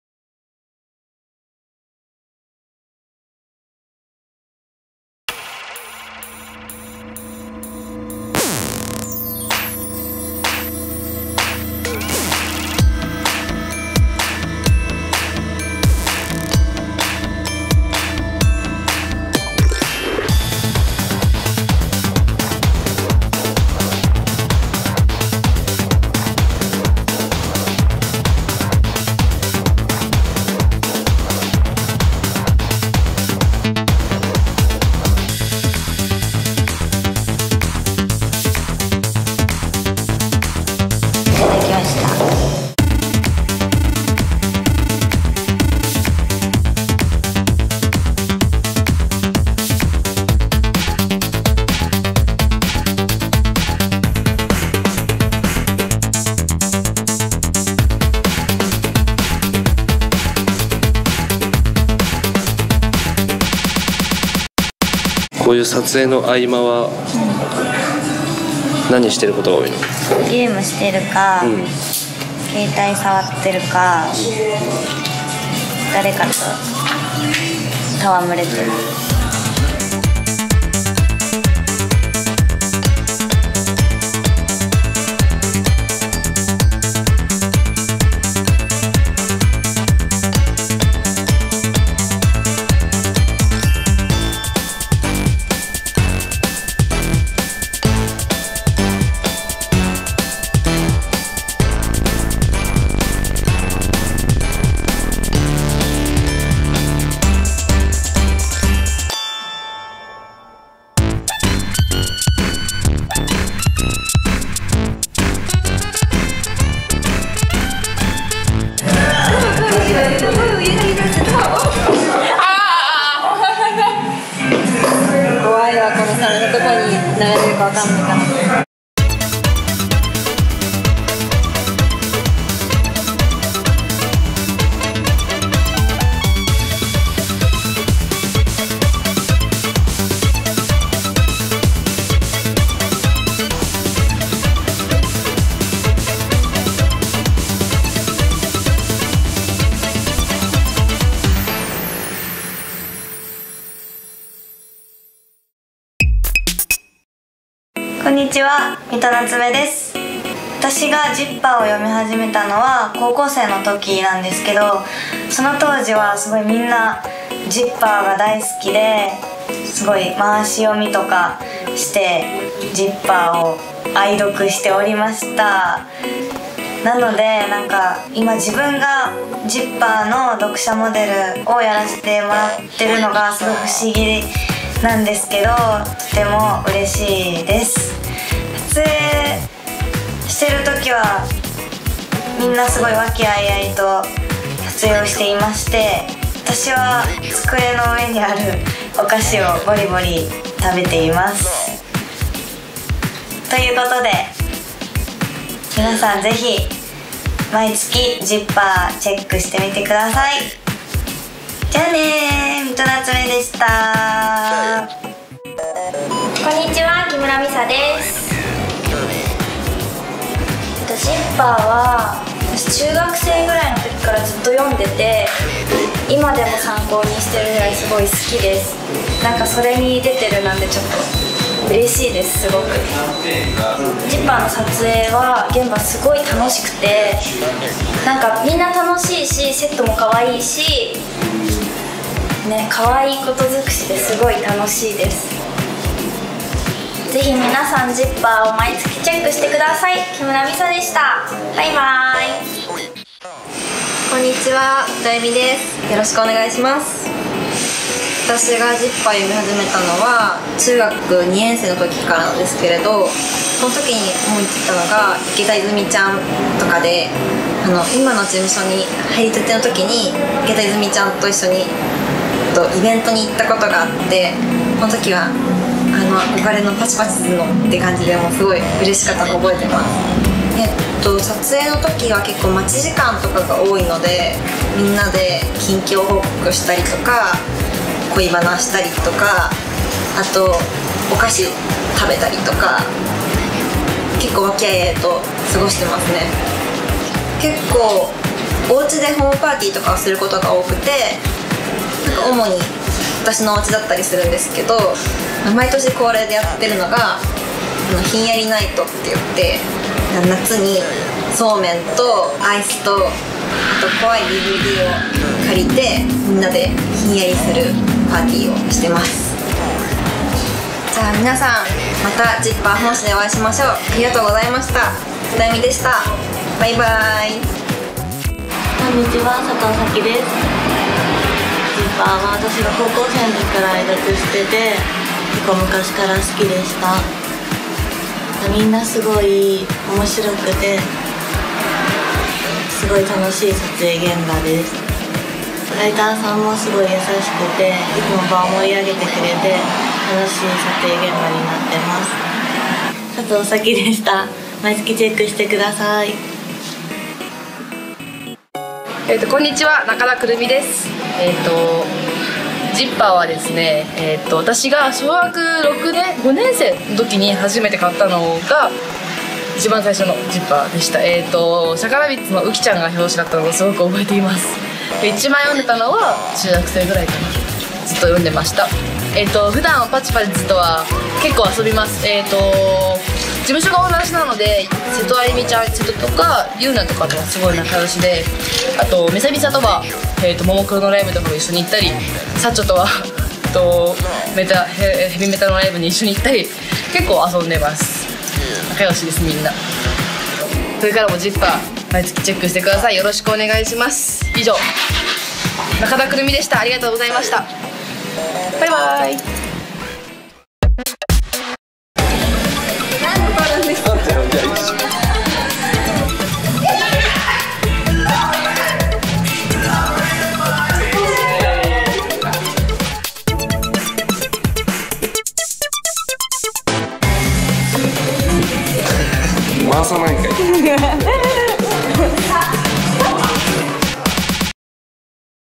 Zipper 9月号撮影舞台裏 │ Zipper Magazine shooting HKTV #090
HARAJUKU KAWAii!! TVオリジナルムービーの第090回放送
#090 HARAJUKU KAWAii!! TV Original Movie

【出演】
アーティスト：きゃりーぱみゅぱみゅ、近藤夏子
モデル：武智志穂、田中里奈、青柳文子、三戸なつめ、瀬戸あゆみ、Una、木村ミサ、中田クルミ­、やのあんな、あぽち

【コーナー】
チェキ散歩 青柳文子、『なつめさん』三戸なつめ │ CHEKI WALKING